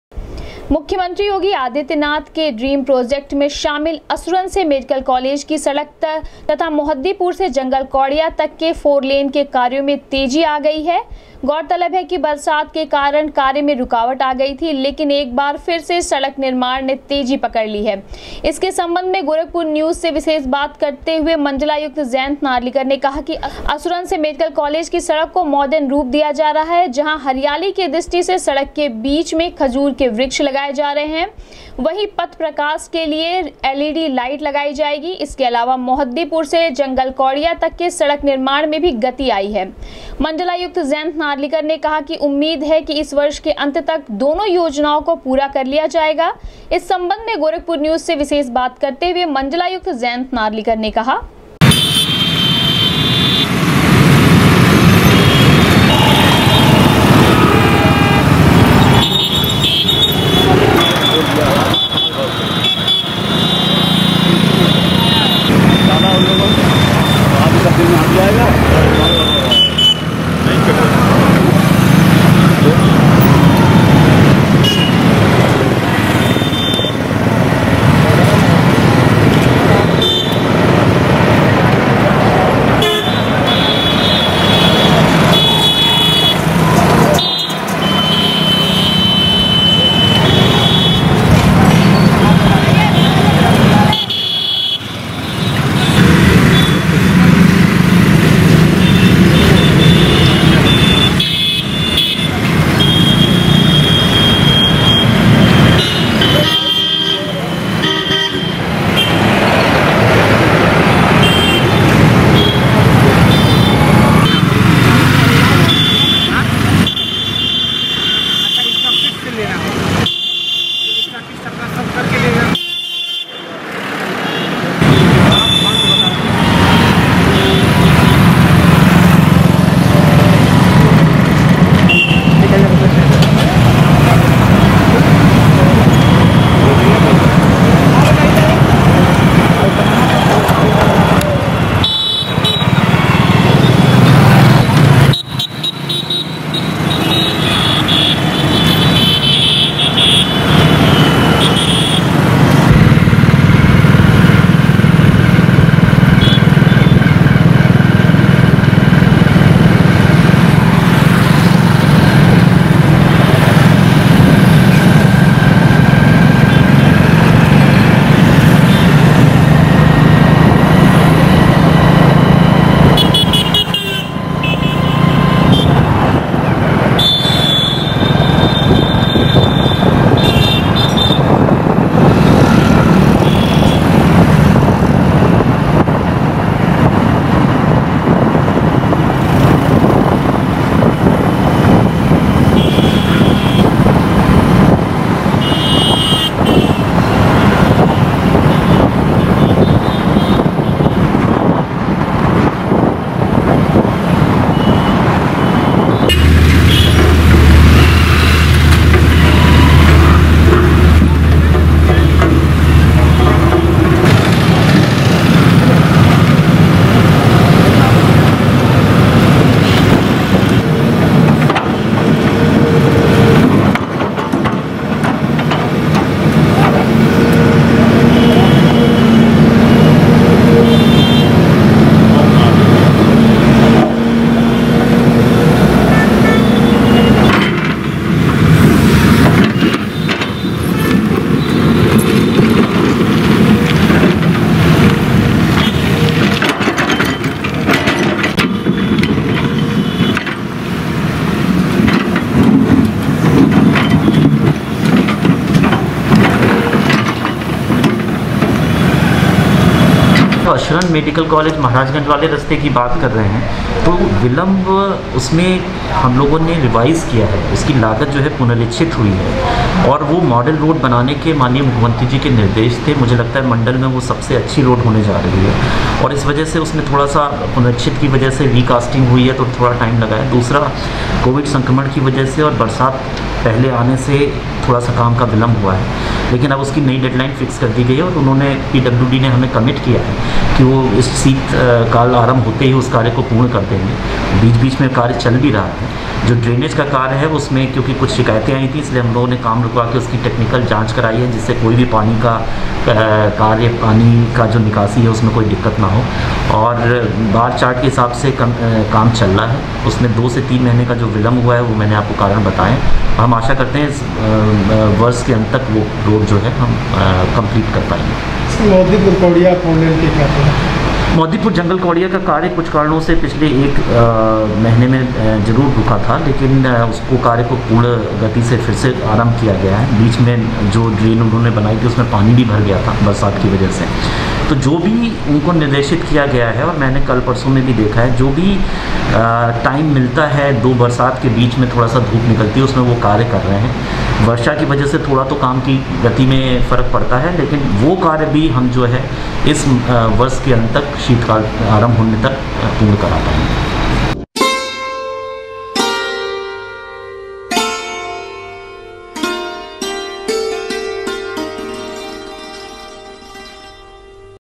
गोरखपुर न्यूज से विशेष बात करते हुए मण्डलायुक्त ने कहा ,असुरन से मेडिकल कालेज की सड़क बनेगा मॉर्डन
गोरखपुर न्यूज से विशेष बात करते हुए मण्डलायुक्त ने कहा ,असुरन से मेडिकल कालेज की सड़क बनेगा मॉर्डन सड़क,गोरखनाथ मन्दिर रोड की सड़क के कार्य की प्रगति हुई तेज,वर्ष के अंत तक दोनो सड़को का होगा निर्माण कार्य पूरा